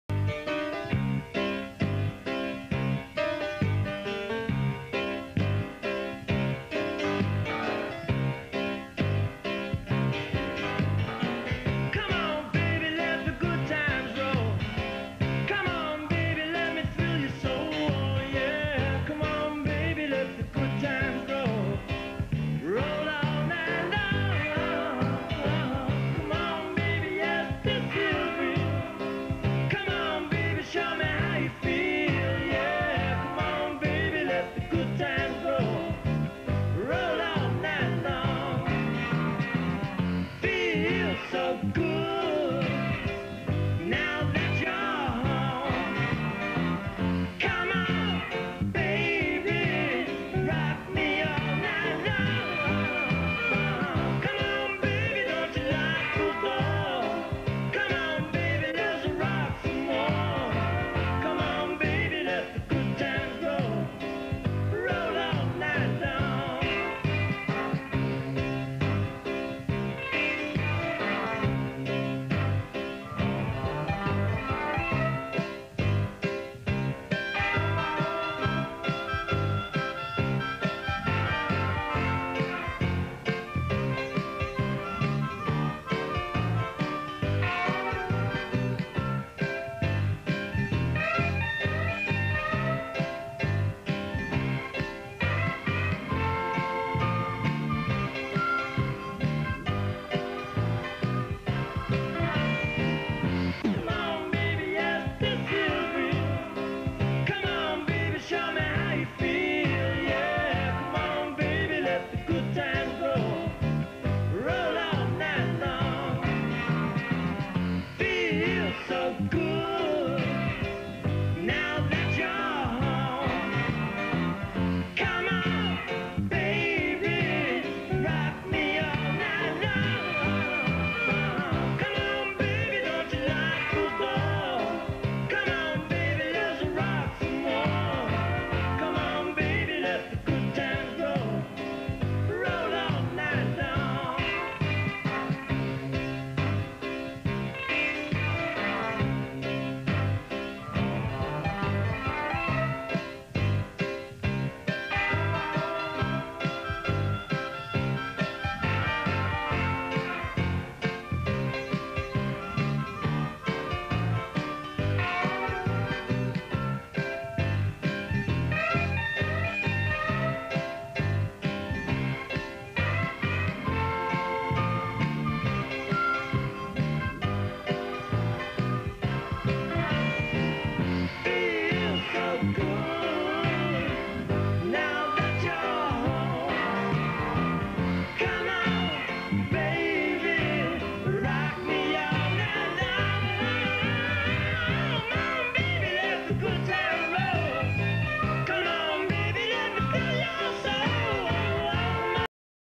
Hell Weekend 1965
Pledges go through the final phase of hazing in order to join a fraternity. (1965)